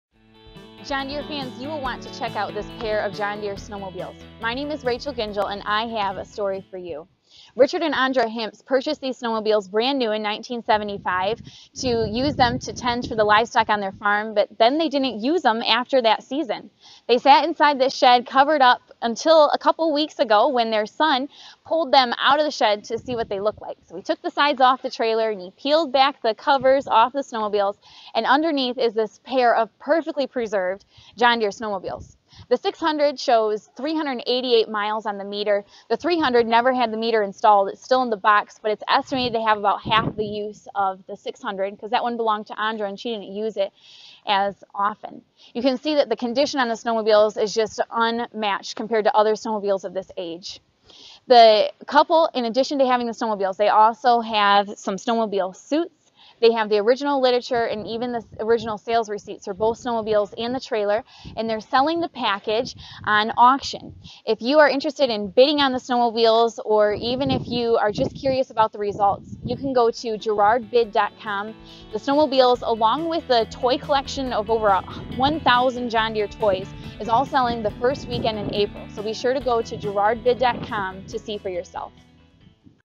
John Deere Snowmobiles: Preserved since 1975
Checkout this phenomenal pair of JD 300 and 600 Snowmobiles. They haven't seen daylight since 1975!